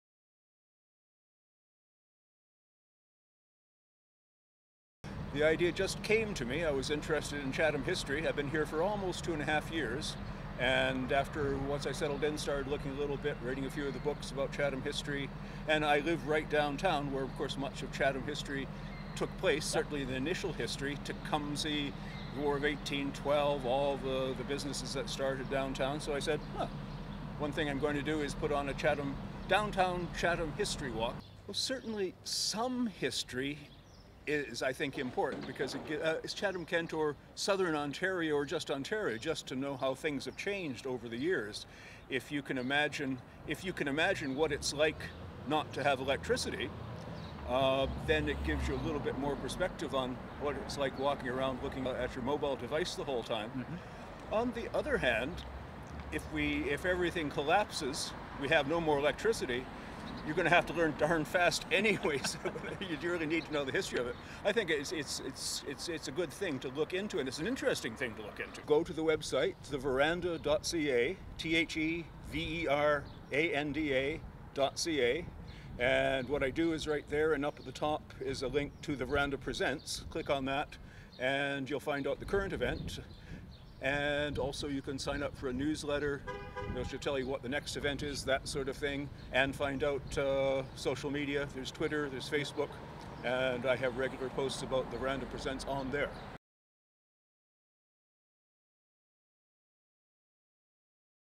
Learning about Chatham's History
Recently, Clair Culliford​ hosted two historic walks around downtown Chatham.

Clair is enthusiastic about sharing stories with the community from the municipality's past, and plans to host other events.

Community Producer: Marlee & Scott Robinson
Edited by: Luke KIm